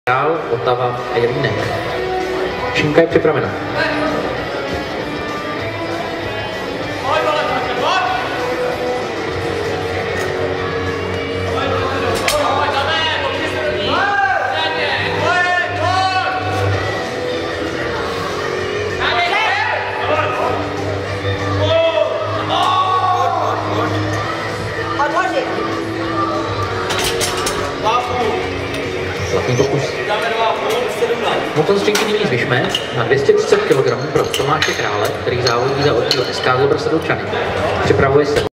Krajdl Filip (1997), DŘ1, 225 kg
5. Mistrovství ČR v klasickém (RAW) silovém trojboji dorostu a juniorů

Místo: Sportovní hala, Praha - Chodov